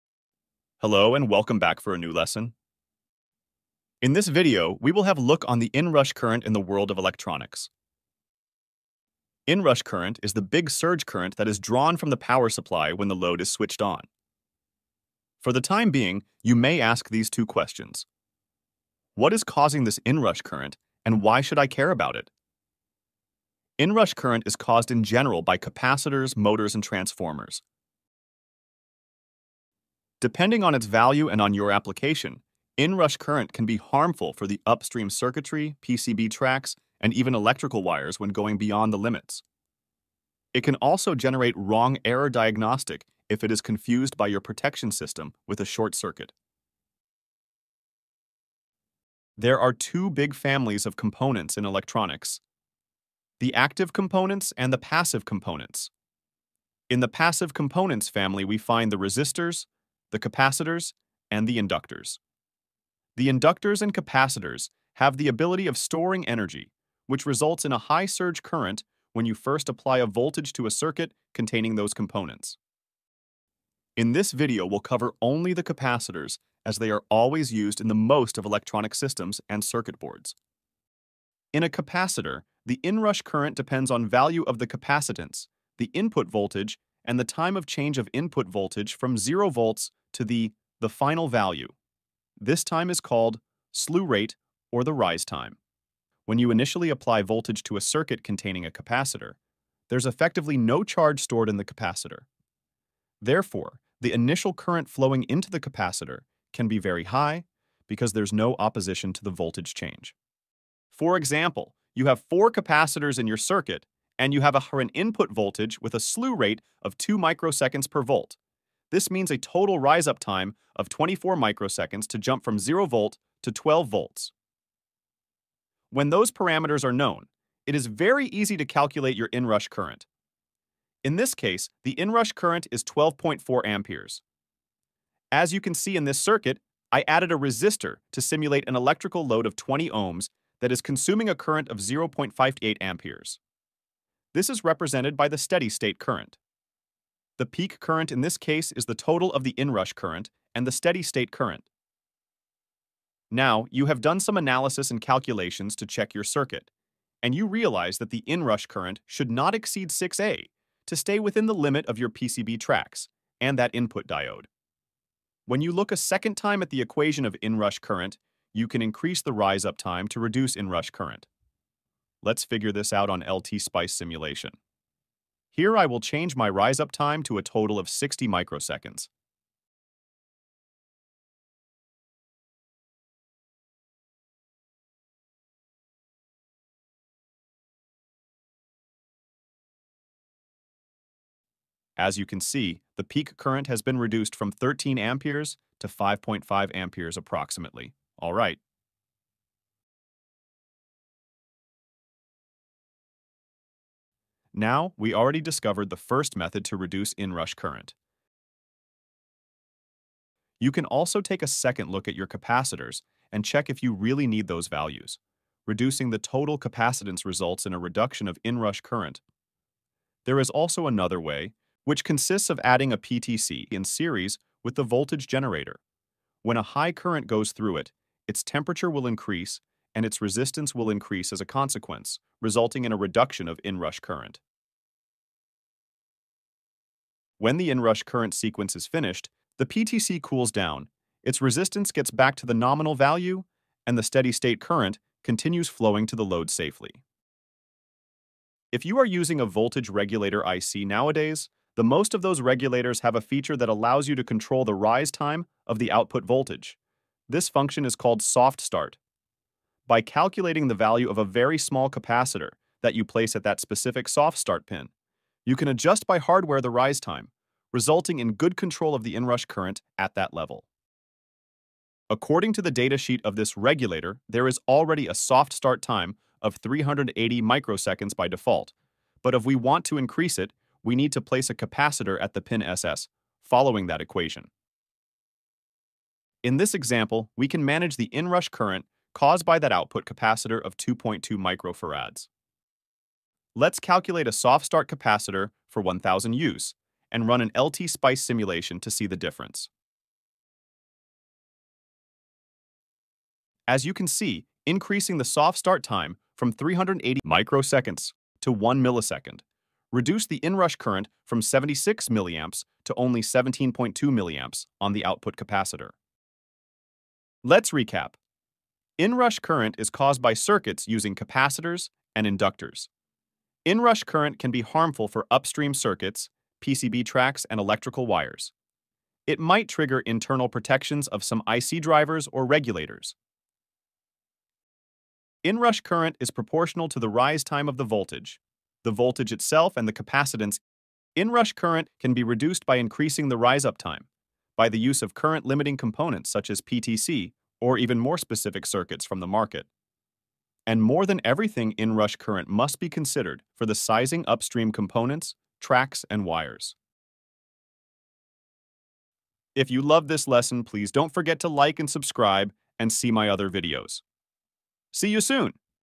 RT#7. Inrush Current and reducing methods - Ltspice Simulation
This video is destinated for engineers and students that would like to learn about inrush current phenomenon faced in electrical engineering.

It is recommended to view my other videos about capacitor charge and discharge if needed to get up to speed.

//1 abstract has been used from the datasheet of ADP7112 from Analog Devices to explain the Soft-Start function

This video does not promote any content or product.